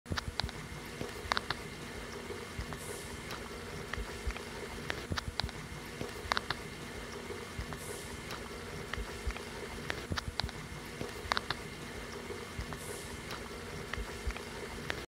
[Houdini] Boiling Liquid in Laboratory
A scene inspired by Tim van Helsdingen's video.

Scene, models, and effects are all constructed in Houdini. Rendered using Redshift.